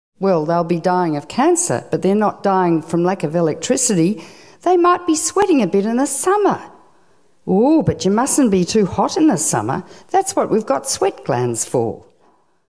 3h24m33s04f Dr. Helen Caldicott Mocking Japan's Lack of Electricity - TR2016a
Well they'll be dying of cancer, but they're not dying from lack of electricity. They might be sweating a bit in the summer. Oooh, but you mustn't be too hot in the summer! That's what we've got sweat glands for. -Dr. Helen Caldicott